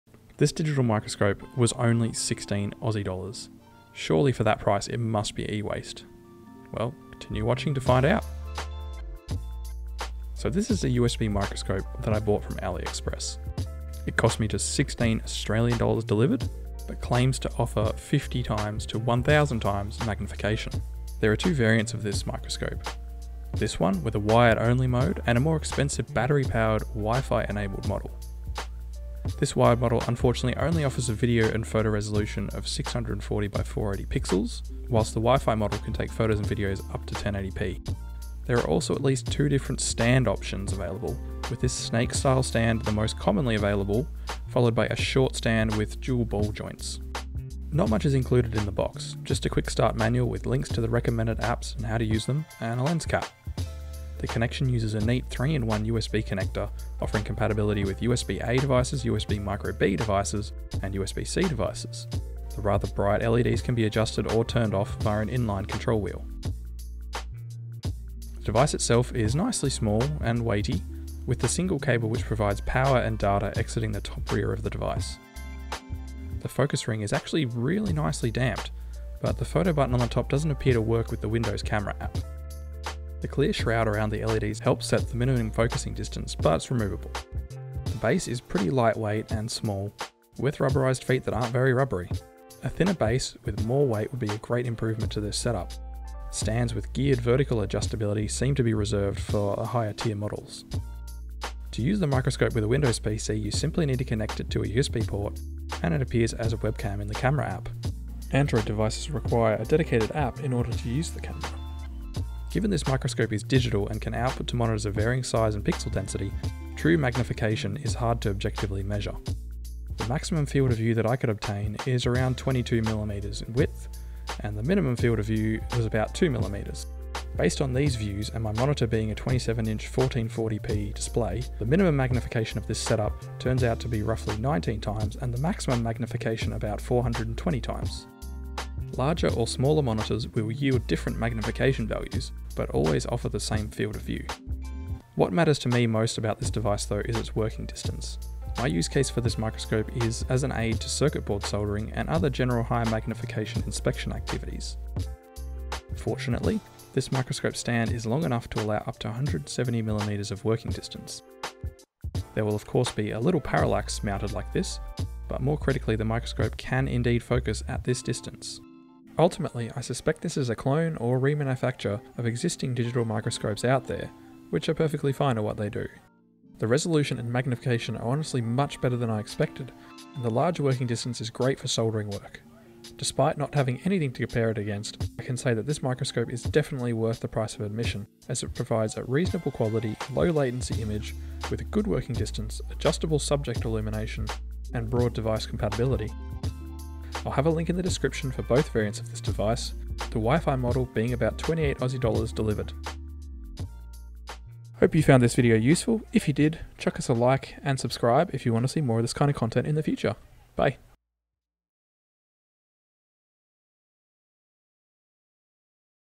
A $16 1000x Digital Microscope?!
Rapid review of the AU$16 AN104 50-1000x, wired, 480p digital microscope from Aliexpress!

The microscope I'm currently using

My tools & gear
Must buy games & gear
My wishlist